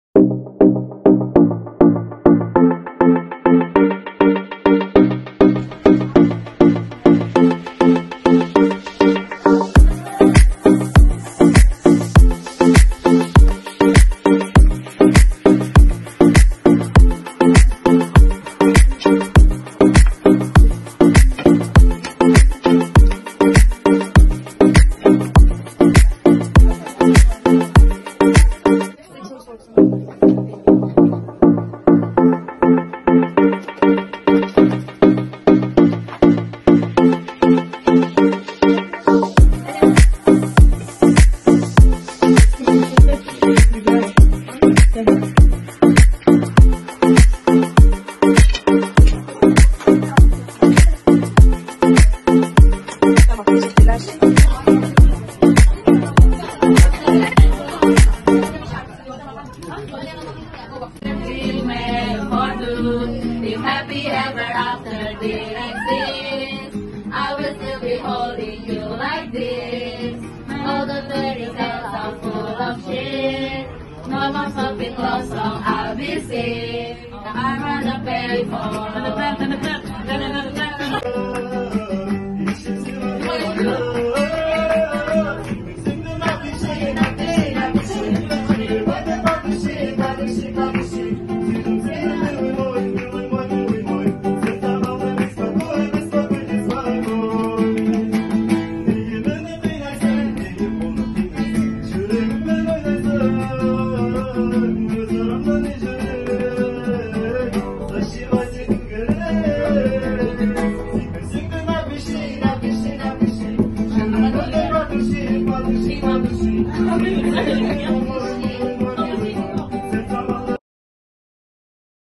Kültürel Uyum Dersi "Türk Mutfağı"
Bu haftaki dersimizde gözlemenin tadına baktık.